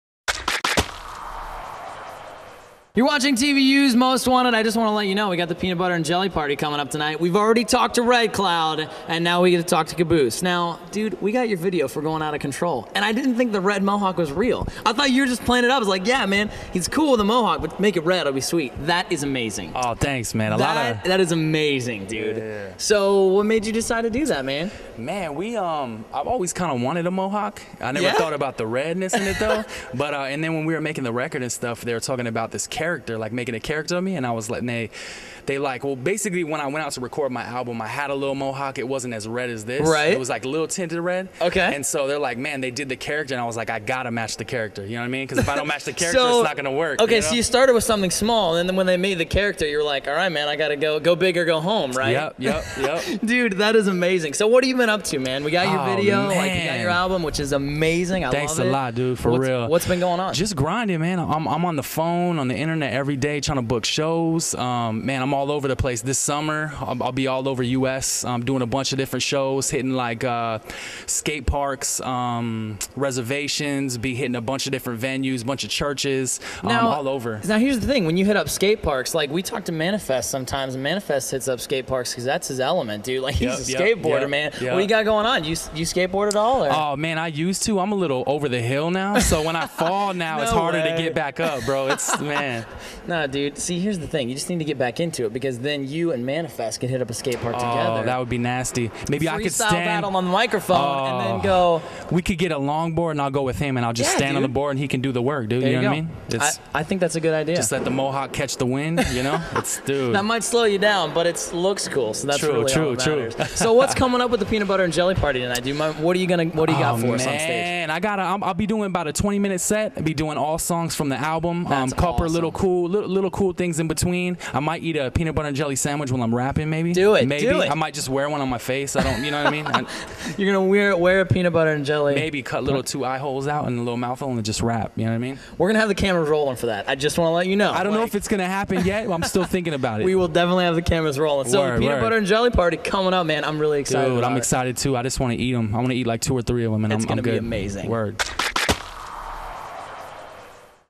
Kaboose Interview at the Peanut Butter & Jelly Party
TVU's Jared interviews Kaboose at the PB&J party